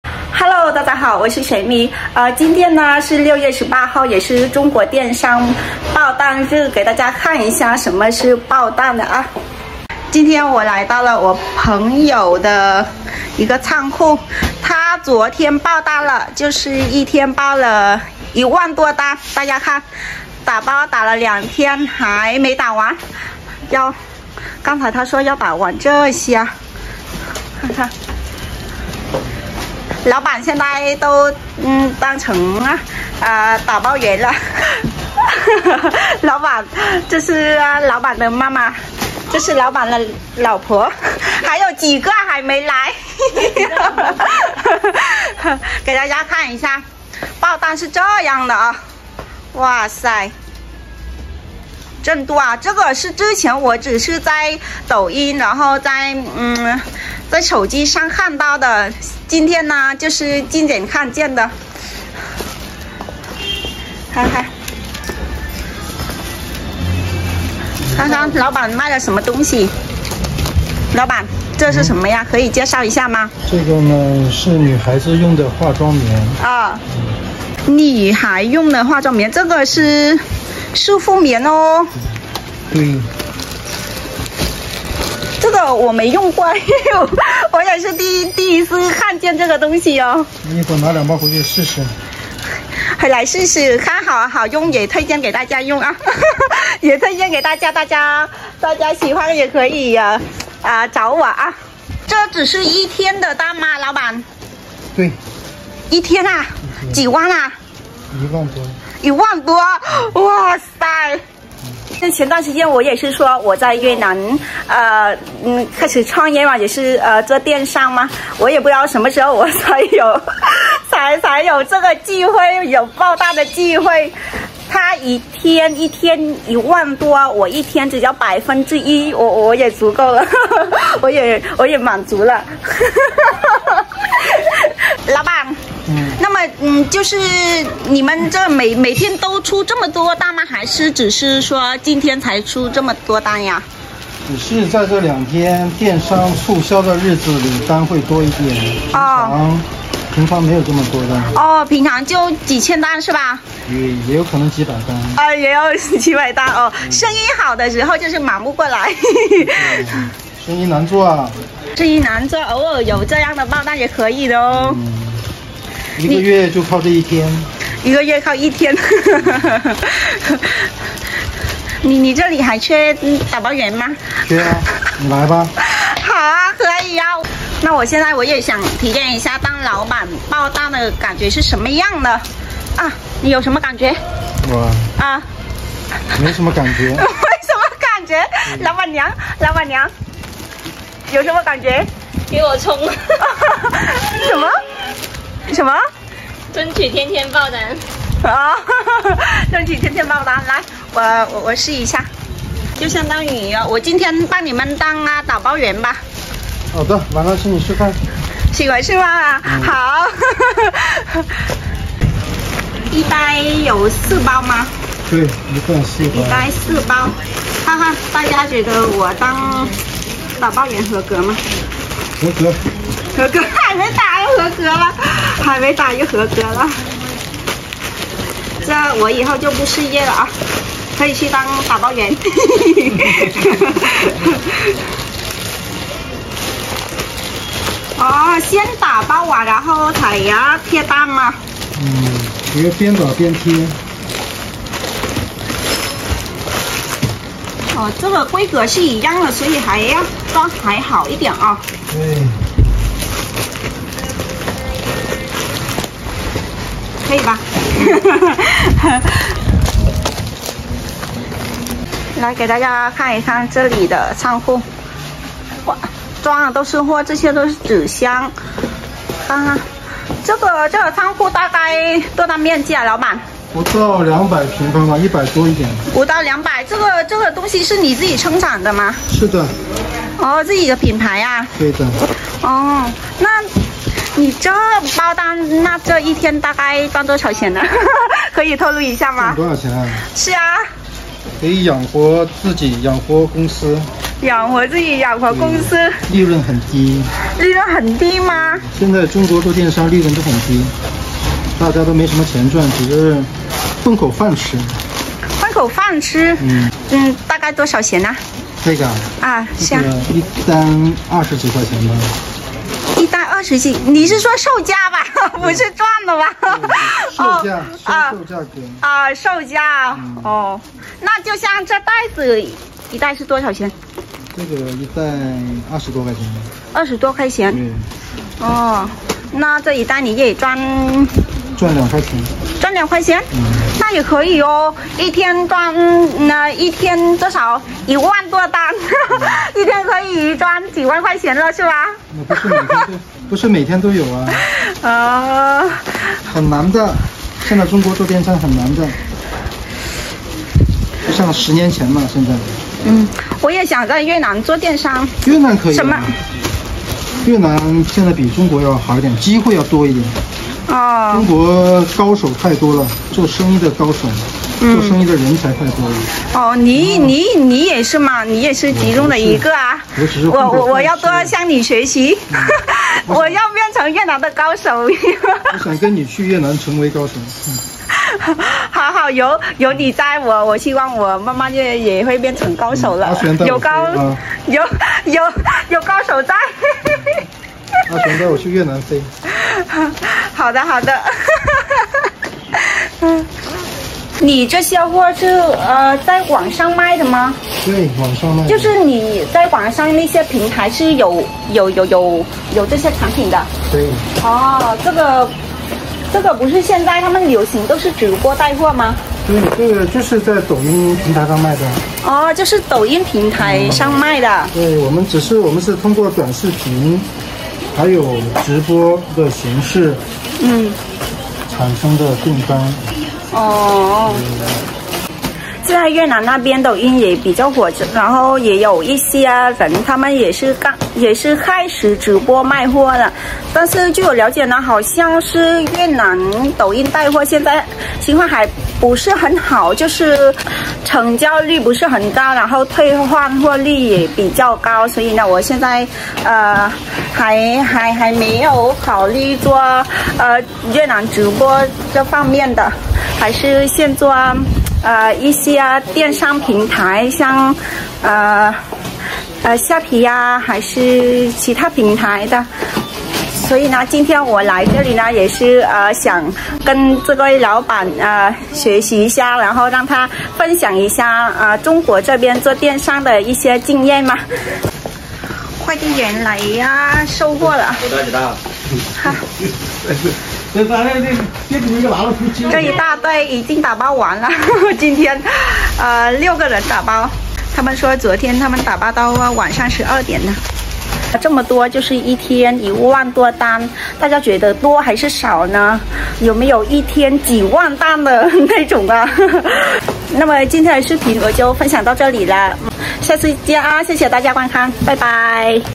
越南媳婦向電商老闆學習，一天爆一萬多單：還是中國消費能力強！
越南姑娘遠嫁河南，分享幸福生活～